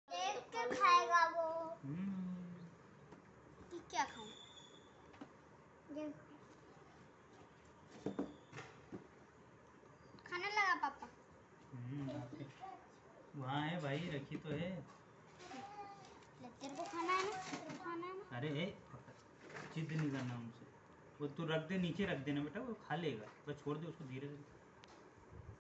guinea pig vedio
guinea pigs bahut hi achha animal hai jo ki aap ke paribar or khas kar aap ke bachcho ke sath bahut hi achhi tarah khelta hai. or bachche bhi ise bahu enjoy karte hai.